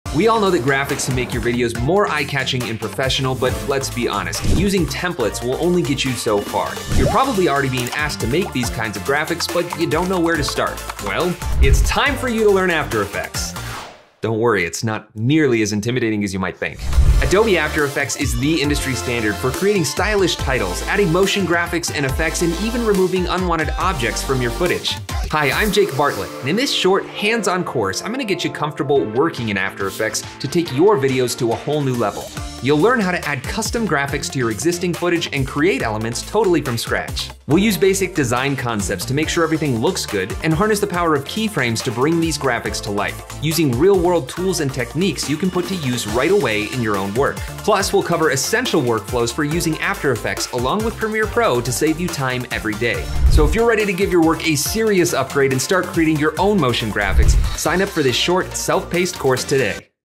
After Effects for Video Editors | Learn After Effects at your own pace
Get comfortable using After Effects for a video editor’s everyday needs in this beginner course from Jake Bartlett. By the end of this course, you’ll be able to create a stylish animated graphics package, remove unwanted elements from footage and track graphics into an existing shot.